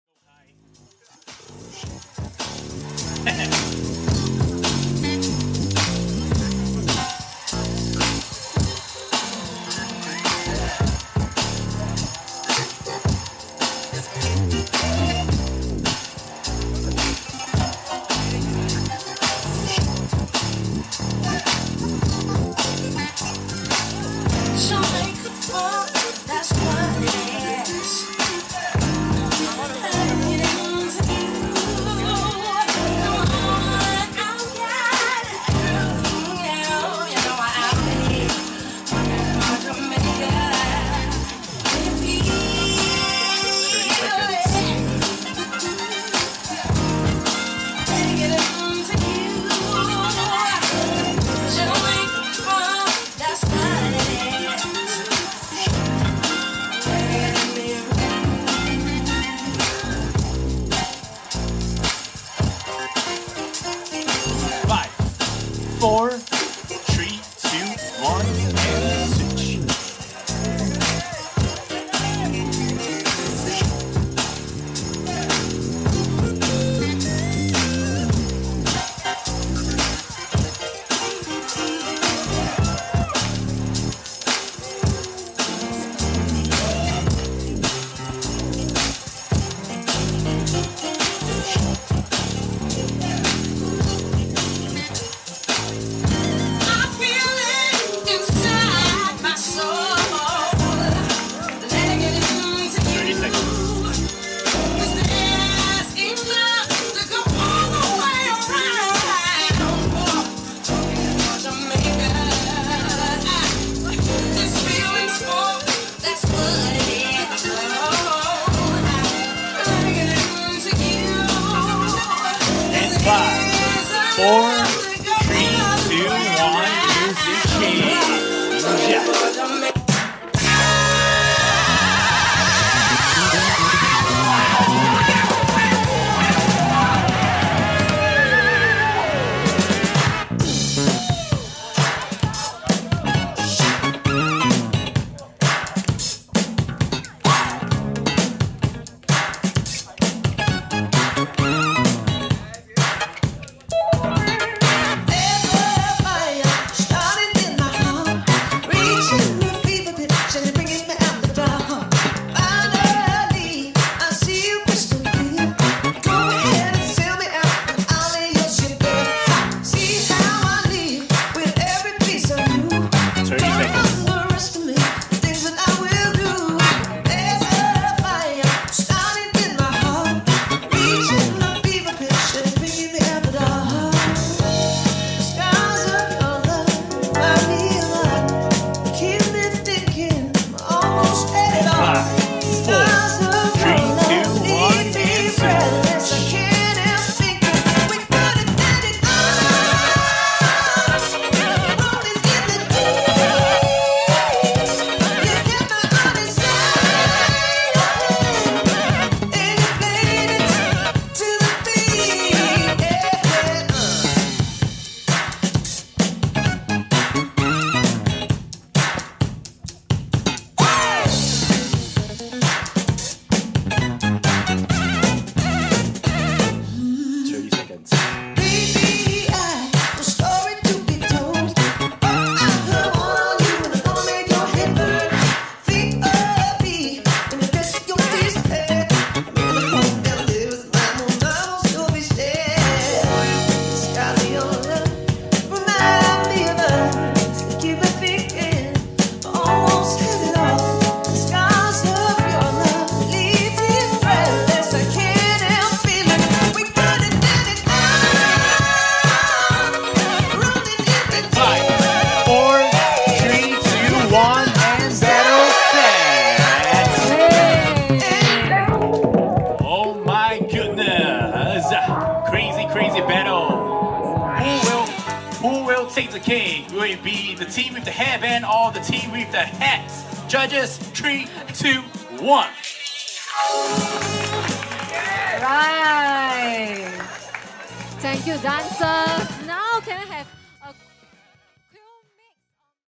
Kevin G vs Egene Melodii | Top 16 | East Side Sessions 2023 Singapore | RPProds
East Side Sessions 2023 Singapore
Organized by Youth Space Online

Judges:
Mandy
Popping KC
Gerald

DJ Raphael
Emcee G Bragon & Agnes

Filmed & edited by Richard Prayoga
Thumbnail Photo by Nicole